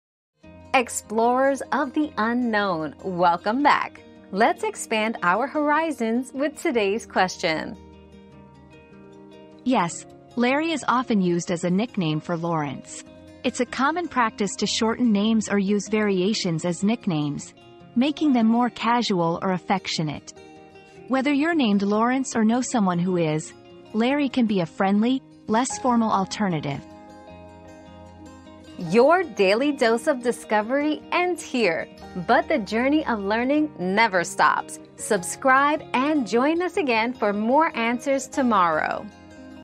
Is Larry short for Lawrence?
Is Larry short for Lawrence? 👉 Larry: A Nickname for Lawrence 👉 Discover whether Larry is a common nickname for Lawrence and how it adds a friendly and casual touch to the name.

Laura S. Harris (2024, January 26.) Is Larry short for Lawrence?
    🌐 AskAbout.video